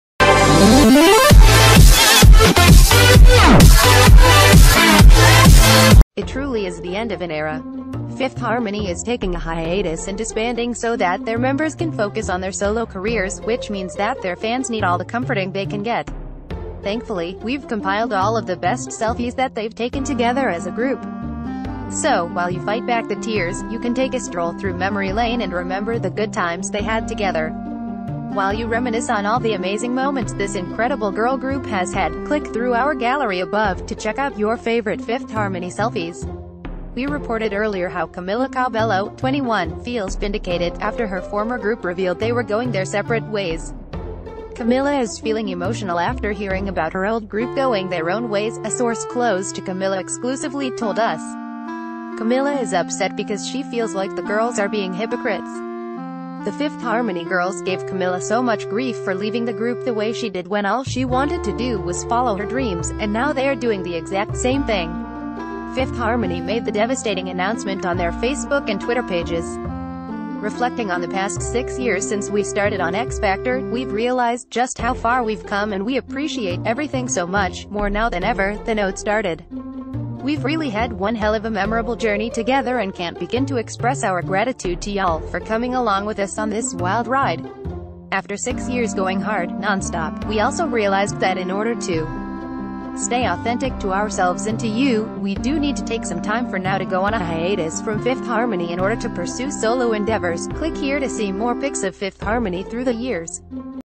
12 Selfies Of Fifth Harmony That Will Make You Miss Them Even More On Hiatus
12 Selfies Of Fifth Harmony That Will Make You Miss Them Even More On Hiatus
It truly is the end of an era. Fifth Harmony is taking a hiatus and disbanding so that their members can focus on their solo careers, which means that their fans need all the comforting they can get! ...